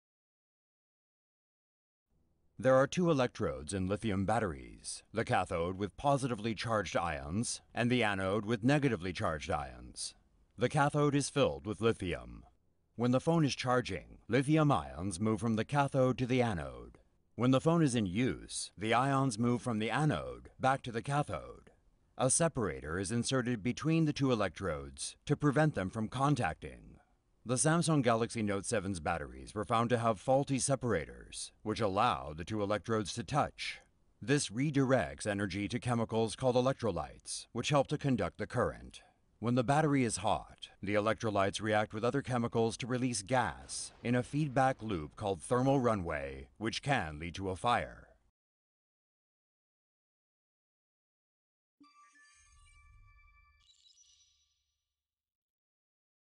Samsung Galaxy Note 7 explodes due to manufacturing error
The US Federal Aviation Administration has advised passengers not to turn on or charge Samsung Galaxy Note 7 smartphones on aircraft after the company admitted that the batteries made by Samsung SDI as the cause of the recent explosions.

There are two electrodes in lithium batteries, the cathode with positively charged ions and the anode with negatively charged ions. The cathode is filled with lithium. A separator is inserted between the two electrodes to prevent them from contacting.

However, the batteries of Samsung Galaxy Note 7 allegedly have faulty separators, which somehow allowed the two electrodes to touch. This redirects energy to the electrolytes, a highly unstable chemical that helps to conduct the current. When the temperature is high, the electrolytes would react with other chemicals to release gas and cause thermal runaway which can lead to a fire.

According to the Wall Street Journal, about 65 percent of the batteries were made by Samsung SDI, while 35 percent were made by Chinese company Amperex Technology. Samsung is recalling about 2.5 million Galaxy Note 7 smartphones globally.